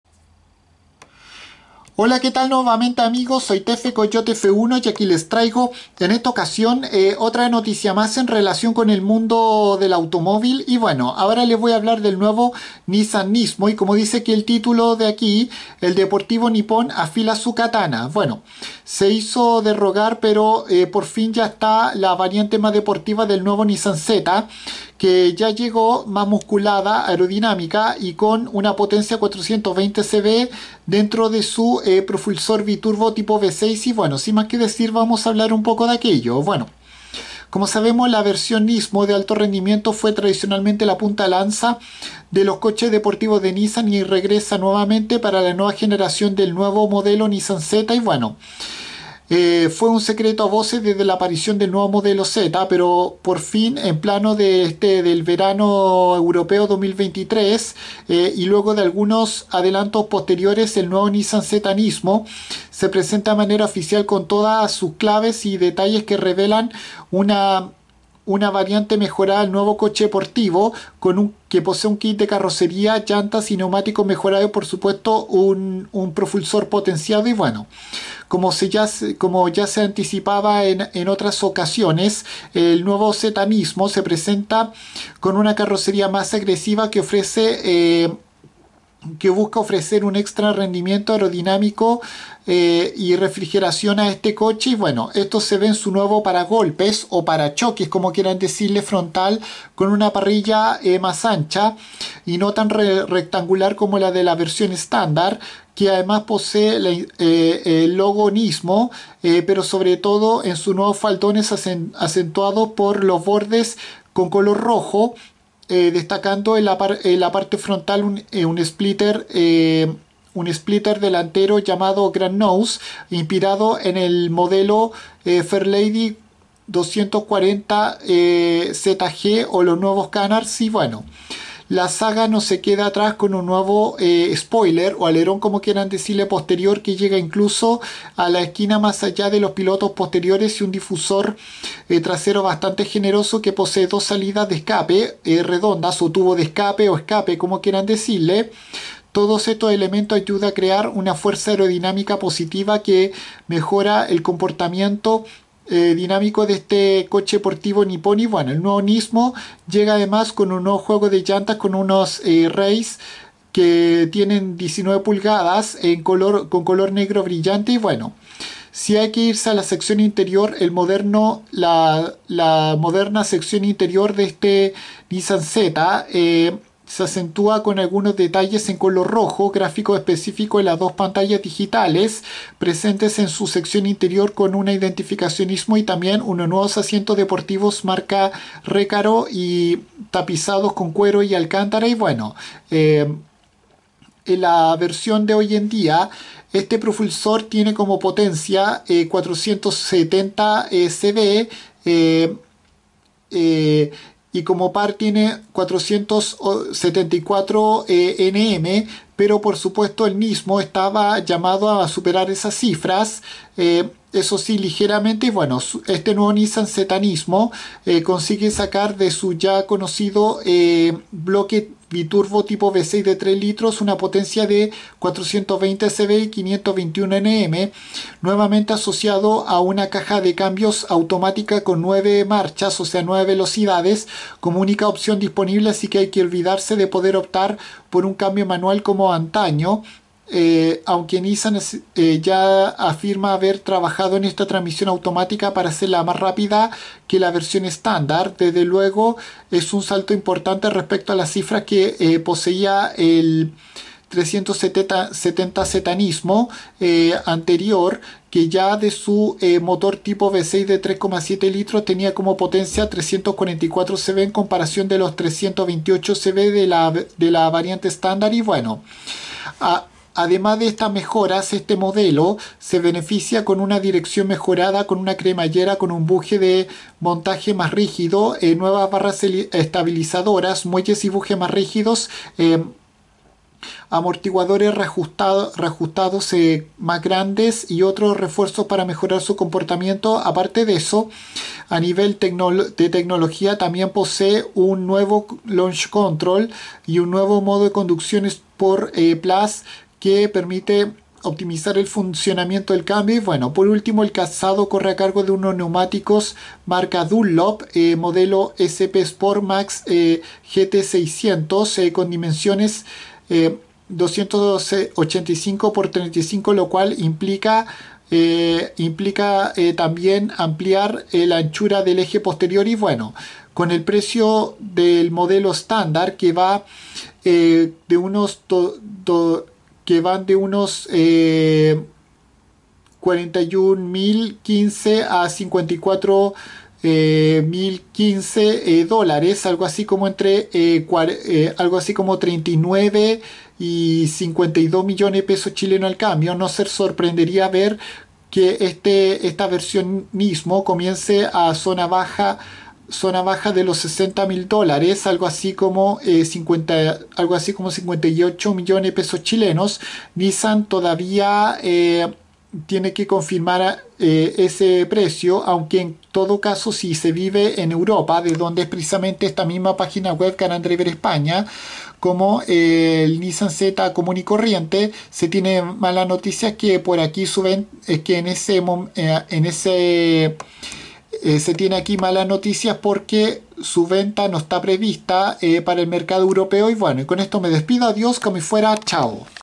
Nissan Z Nismo información
Esta es la nueva variante NISMO de este coche deportivo nipón que ya fue recientemente presentada